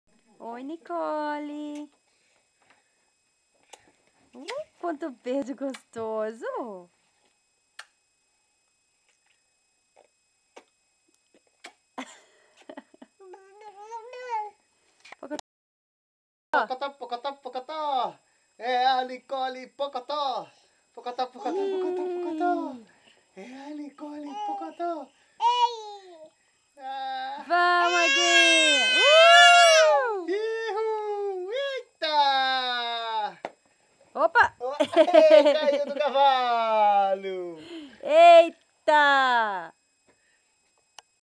Nicolle no pocoto!
Mandando beijos e, no final, caindo do cavalo.
Fev/2009